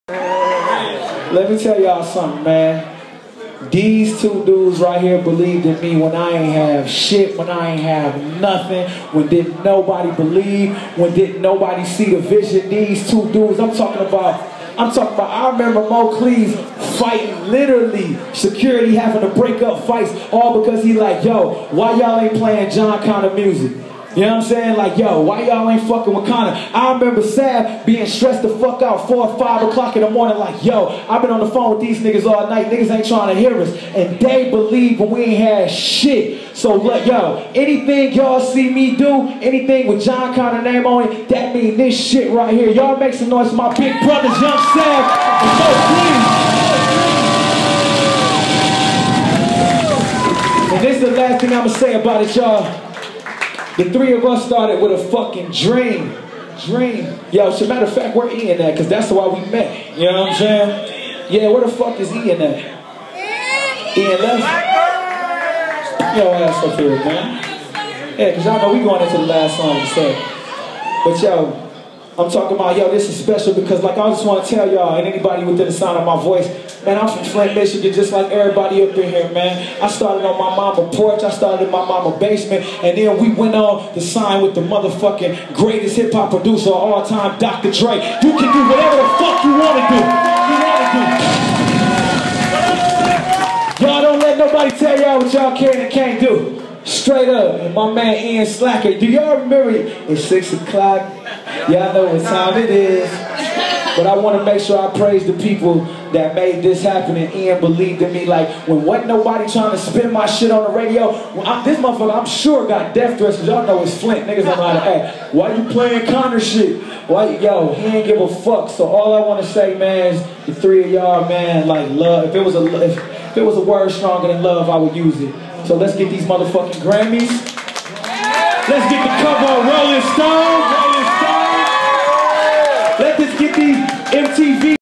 Jon Connor thanks Ian Slacker, Mateen Cleaves, and Young Savv
December 22, 2013. Aftermath's Jon Connor thanks the people who helped him get to where he is now. Ian Slacker, played Connor on the radio when nobody else would. Connor mentions their intro to the six o'clock show Slacker had on Flint's Club 93.7. Mateen Cleaves and Young Savv helped produce and get Jon Connor seen. Connor loves the city of Flint, and his fans. Congratulations Jon, you did it! Blue City Club, youuuu knowwww!